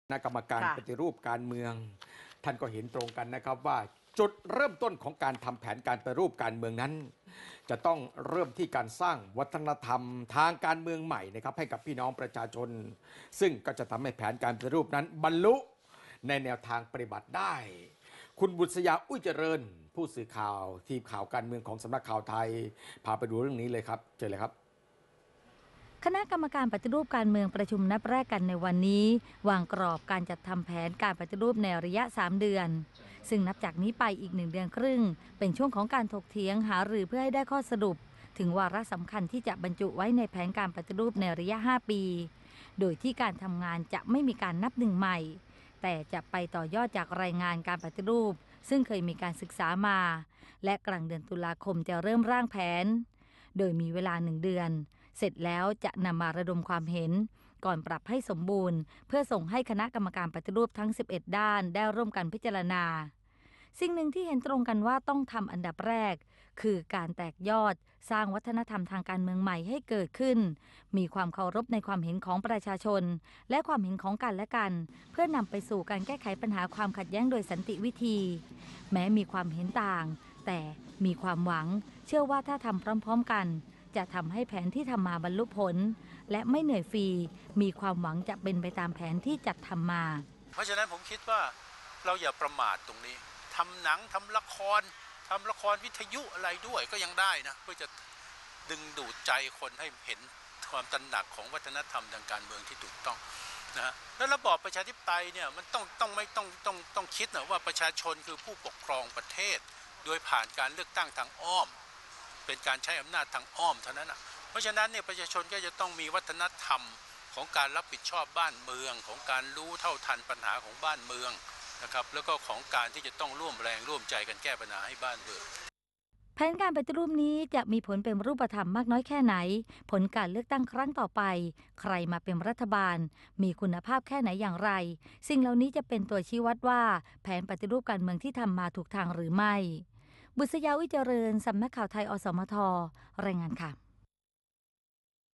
สร้างวัฒนธรรมการเมืองใหม่ ปฏิรูปประเทศ
คณะกรรมการปฏิรูปการเมืองเห็นตรงกันว่า จุดเริ่มต้นของการทำแผนปฏิรูปการเมือง ต้องเริ่มจากการสร้างวัฒนธรรมทางการเมืองใหม่ จึงจะทำให้แผนปฏิรูปการเมือง บรรลุผลในทางปฏิบัติได้

1.สารคดีเฉลิมพระเกียรติ
2.พระราชกรณียกิจ-พระมหากรุณาธิคุณ
3.คนไทยหัวใจ♥รักในหลวง